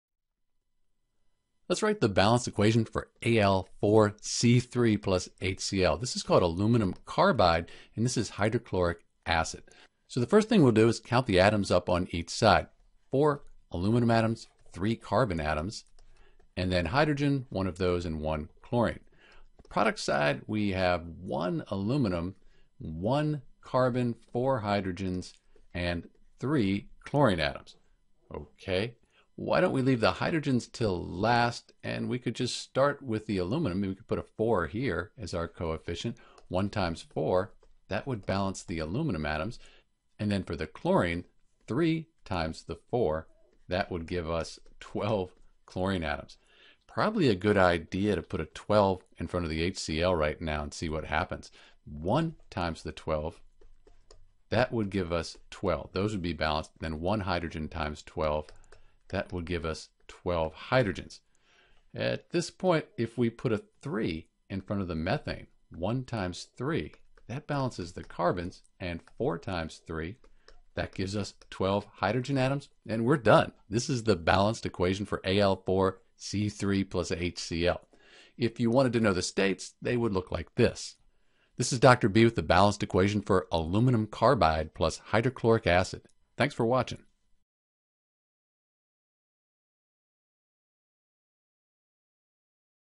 How to Balance Al4C3 + HCl = AlCl3 + CH4
In this video we'll balance the equation Al4C3 + HCl = AlCl3 + CH4 and provide the correct coefficients for each compound.

To balance Al4C3 + HCl = AlCl3 + CH4 you'll need to be sure to count all of atoms on each side of the chemical equation.

Once you know how many of each type of atom you can only change the coefficients (the numbers in front of atoms or compounds) to balance the equation for Aluminum carbide + Hydrochloric acid .